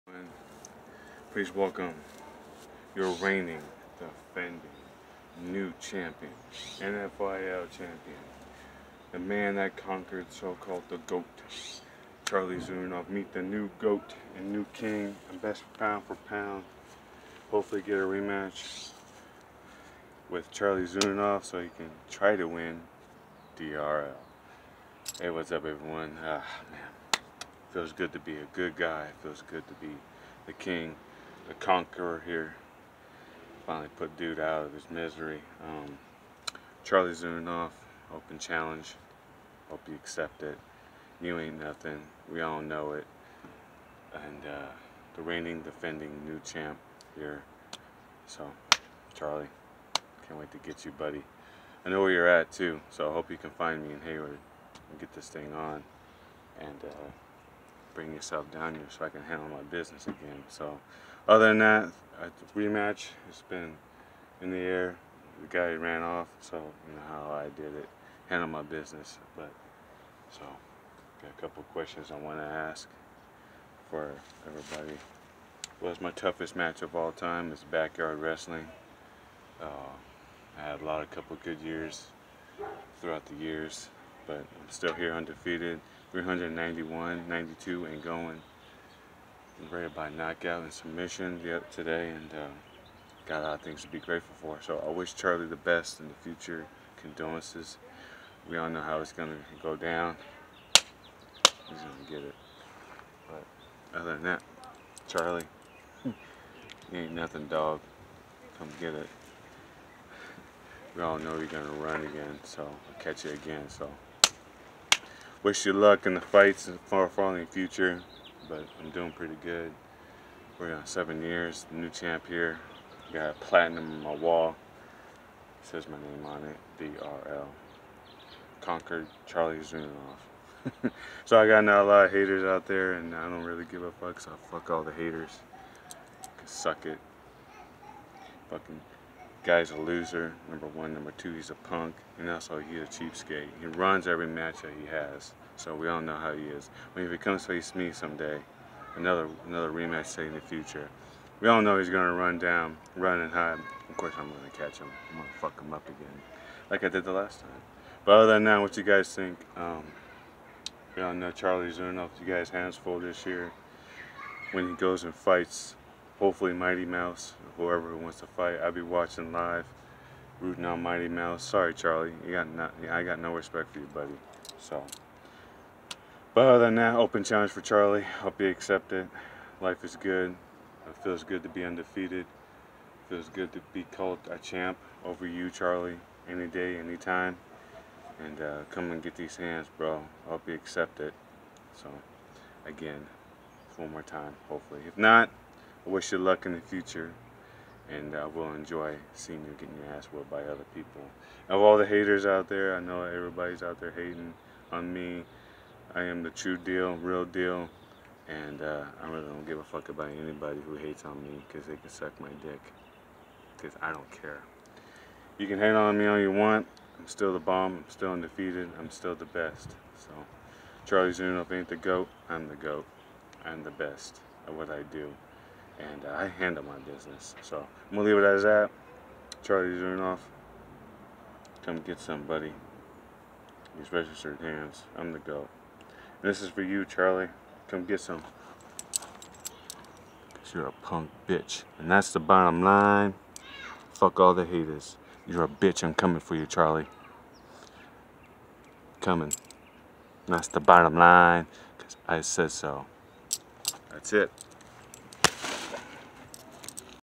The legend interview of new champion 2020
The legend vs Charlie Zelenoff ( DRL new champion unbelievable 392 and 0 2 win over Charlie Zelenoff by way TKO )